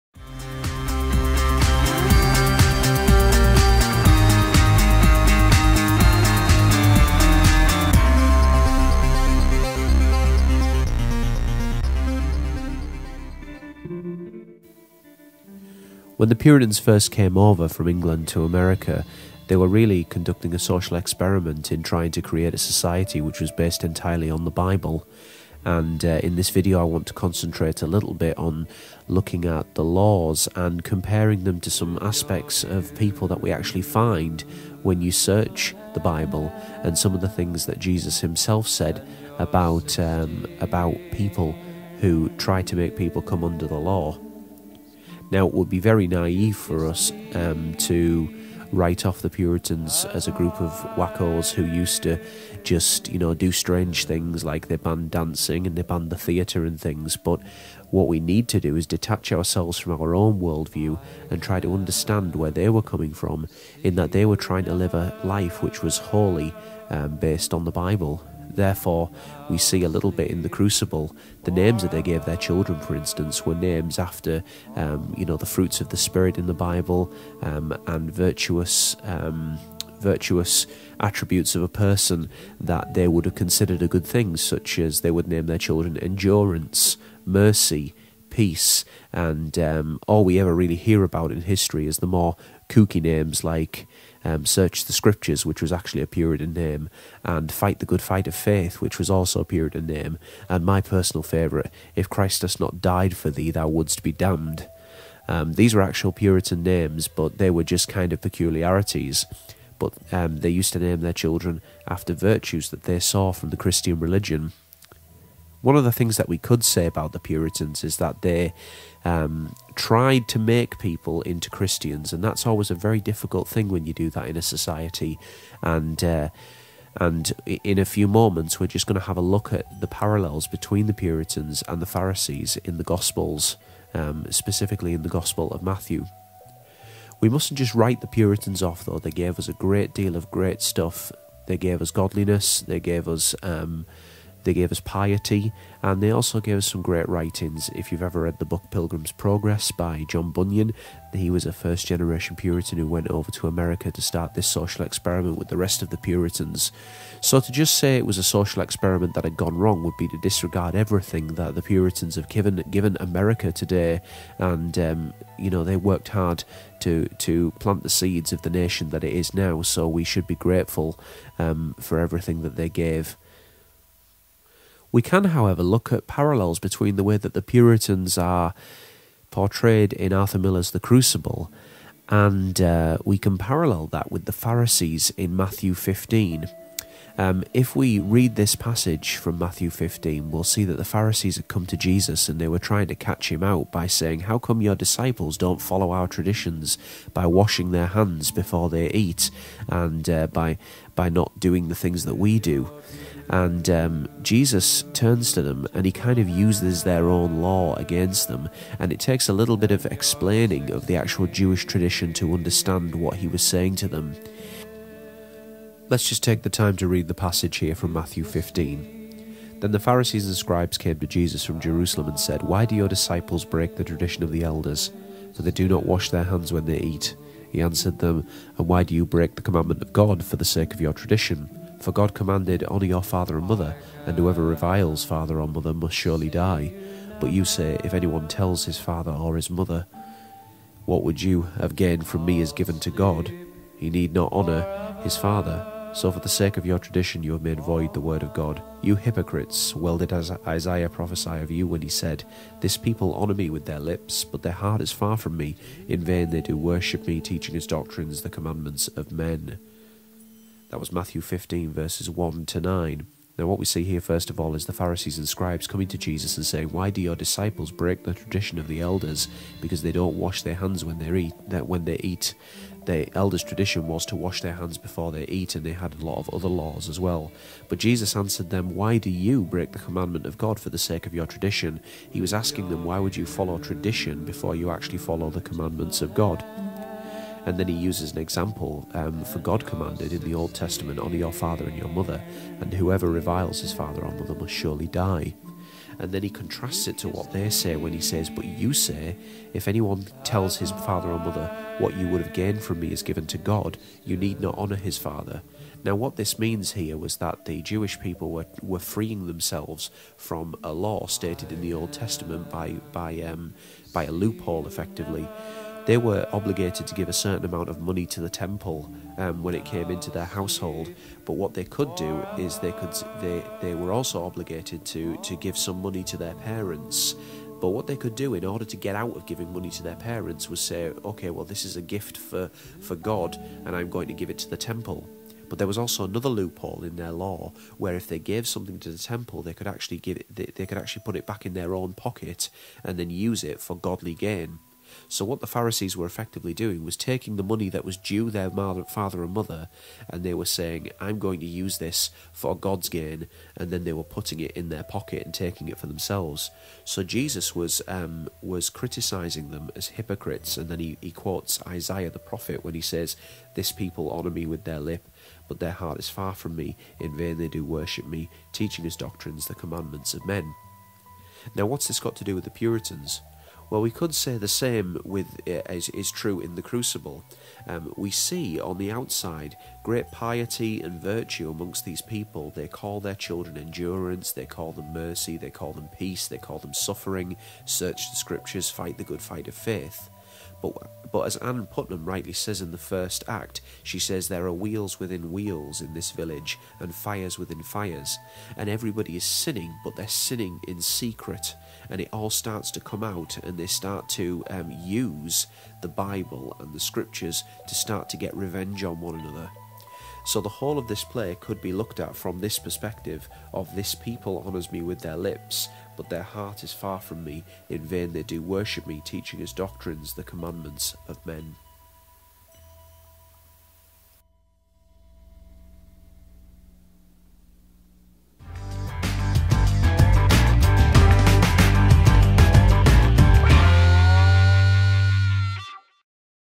The Puritans and The Pharisees
A comparison of the Puritans with the Pharisees in Matthew 15.